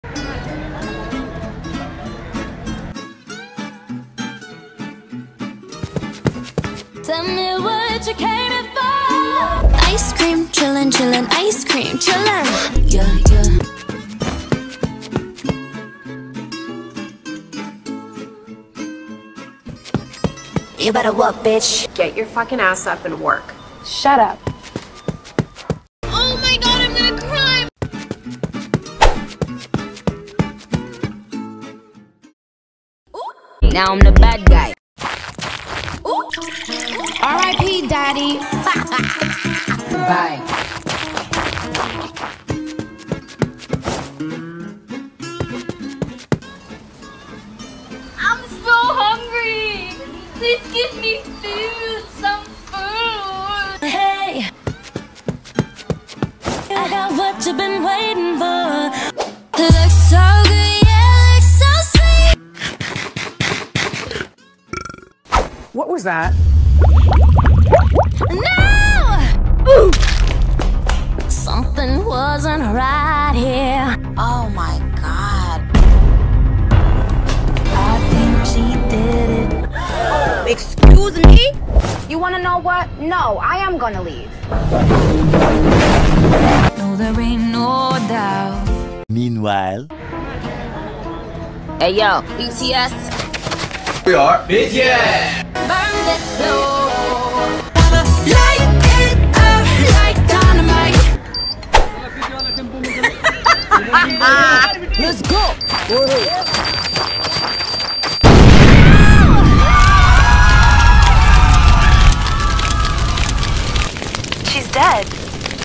Celebrities at Baskin Robbins
So our celebrities at back this time at Baskin Robbins with Ariana Grande being the worker and BLACKPINK coming to get their ice cream but Britney Spears and Kim Kardashian are work buddies but Ari is savage. Nicki is the bad guy with BTS lighting this as playdate

KLS Kingdom is animation Fandom
Time stops when you binge KLS videos... Like literally